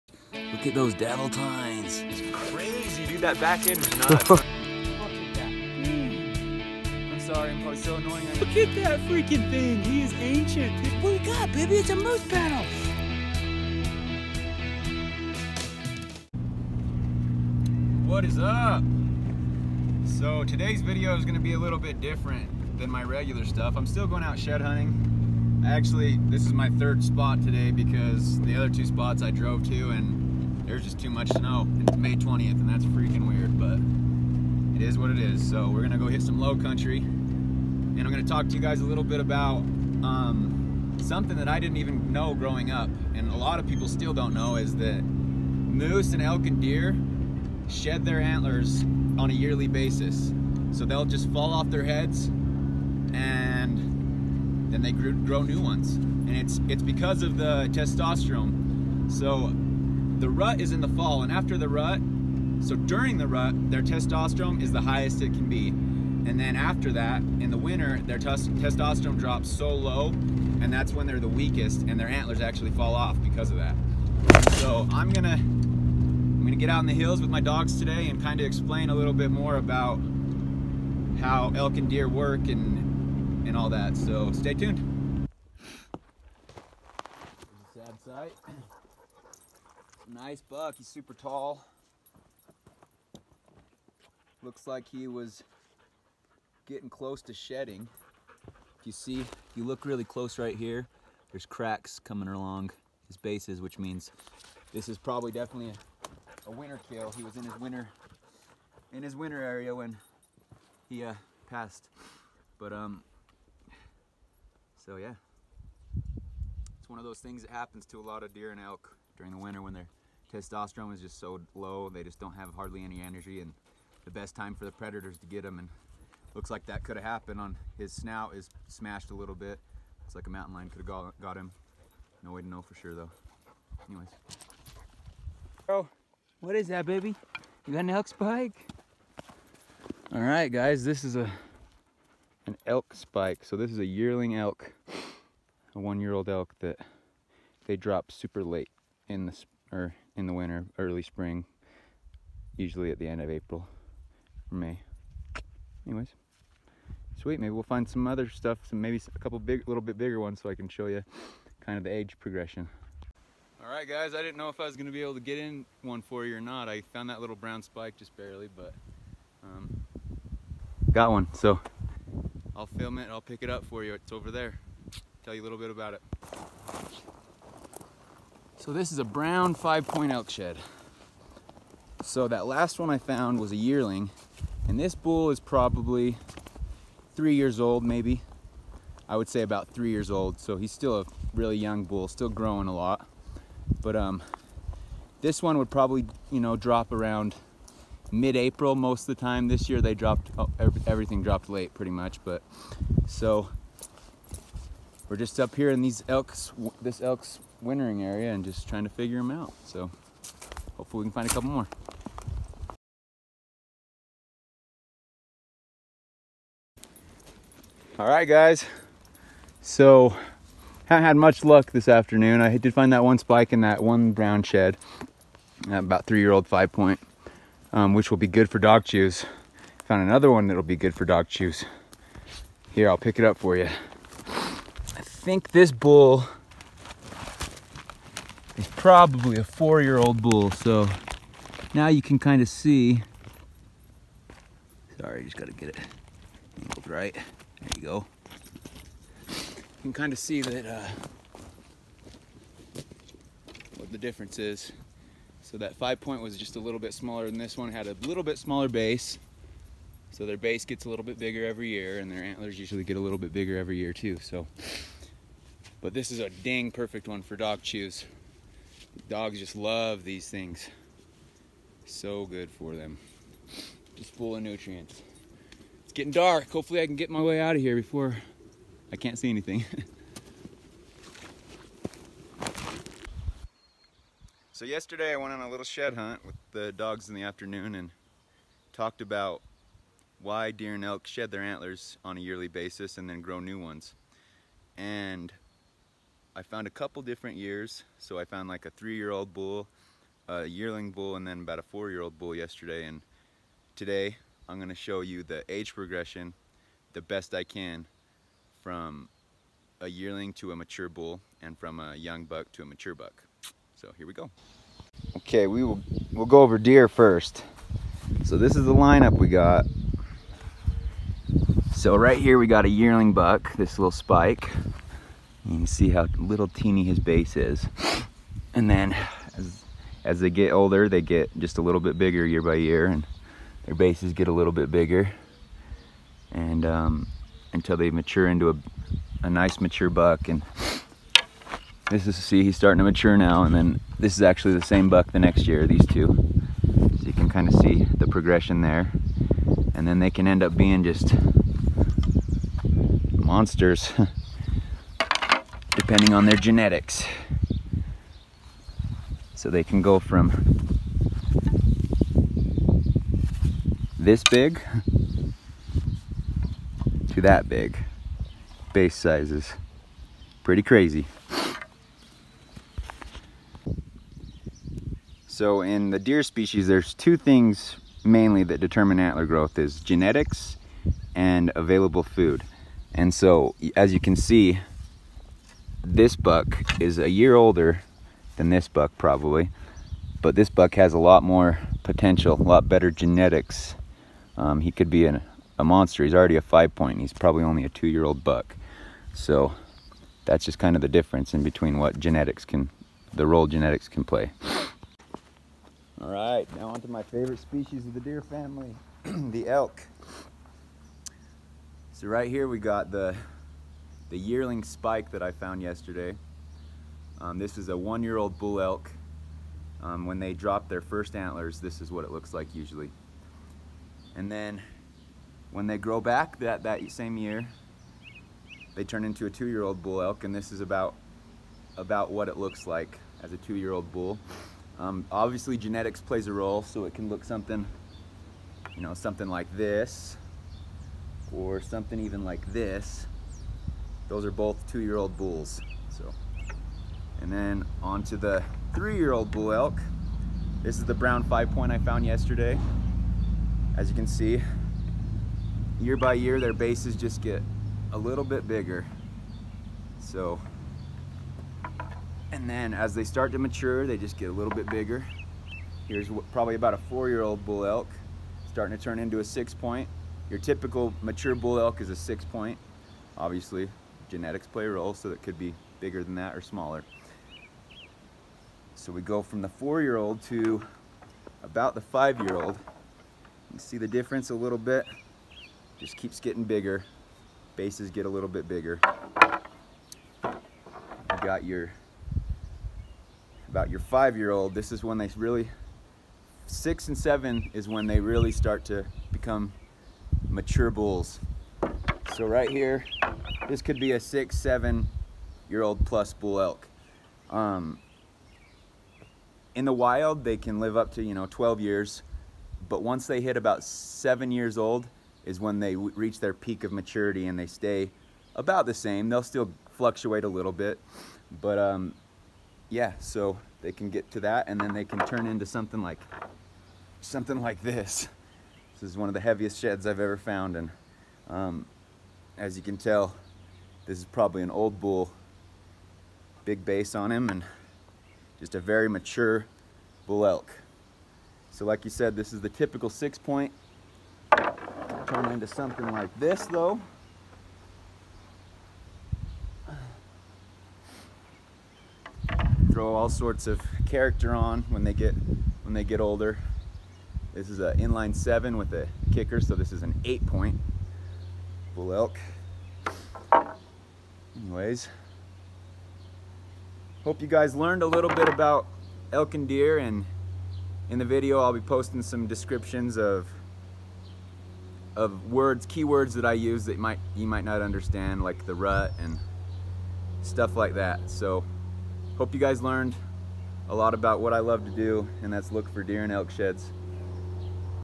Did you know Deer and Elk antlers fall off every year?
Members of the Deer family shed and regrow their antlers every year, heres why.